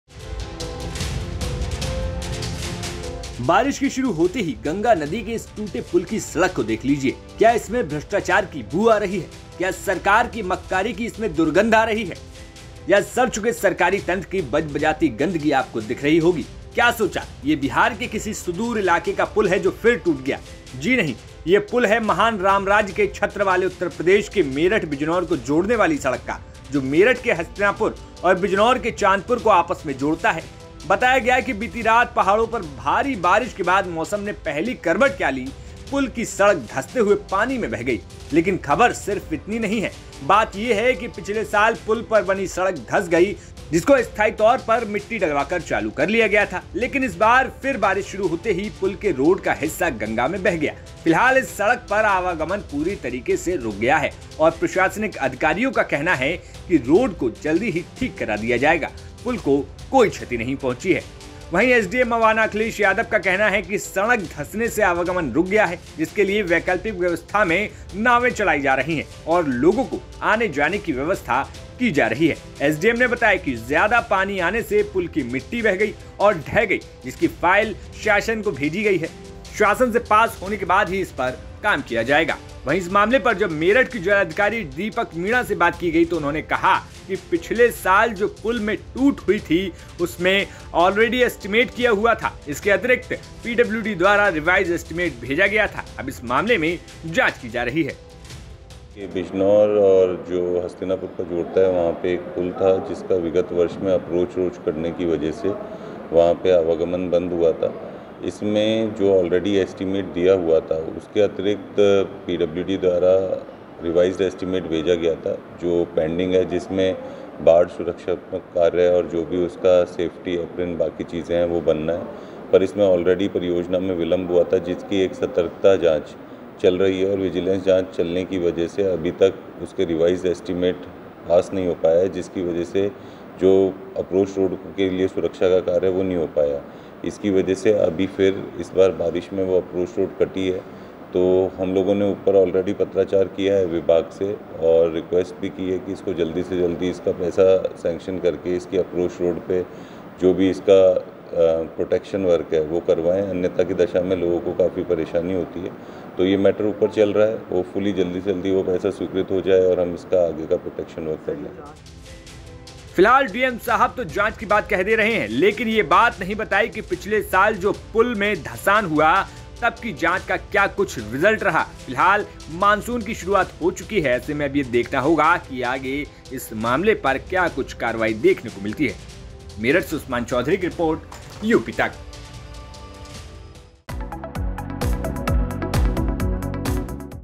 Meerut Viral News : जैसे ही हुई पाहली बारिश पानी में मिला गया ये पुल?
Meerut Viral News : ये पुल ये महान रामराज्य के छत्र वाले उत्तर प्रदेश के मेरठ बिजनौर को जोड़ने वाली सड़क का है. जो मेरठ के हस्तिनापुर और बिजनौर के चांदपुर को आपस में जोड़ता है. बताया गया कि बीती रात पहाड़ों पर भारी बारिश के बाद मौसम ने पहली करवट क्या ली पुल की सड़क धंसते हुए पानी में बह गई. लेकिन खबर सिर्फ इतनी ही नहीं है. क्या है पुरा मामला जाने इस वीडियो में ..